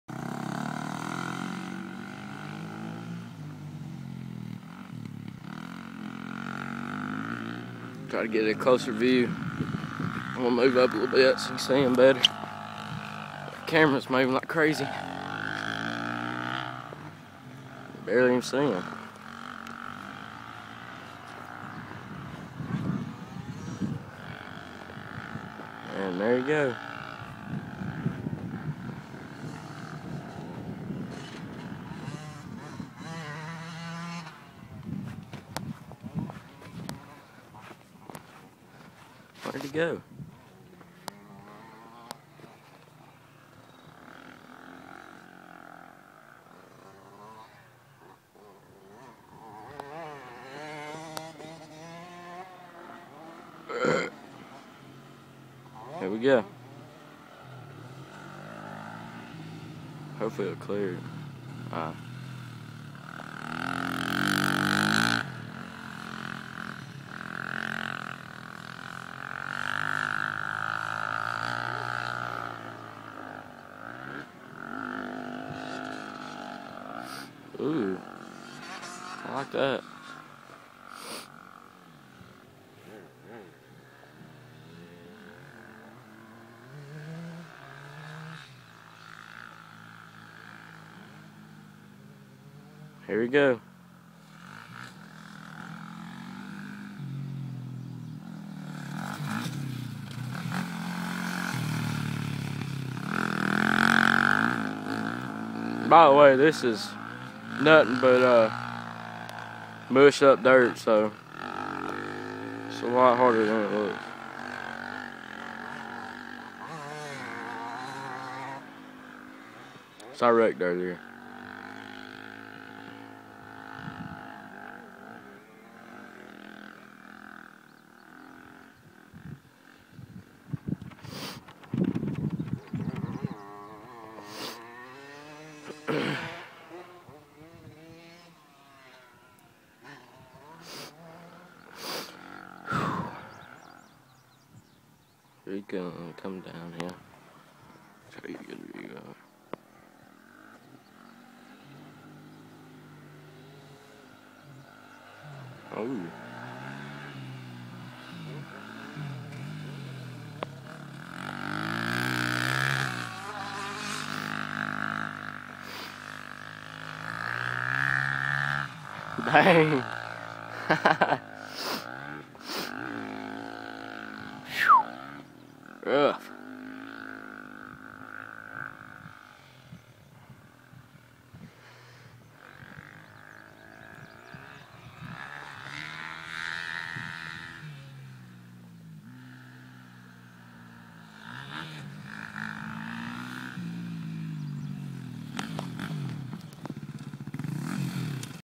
Highland Park Resort MX track 2 Jason Stargell
Alot of thick dirt was down, trying to make a line to ride.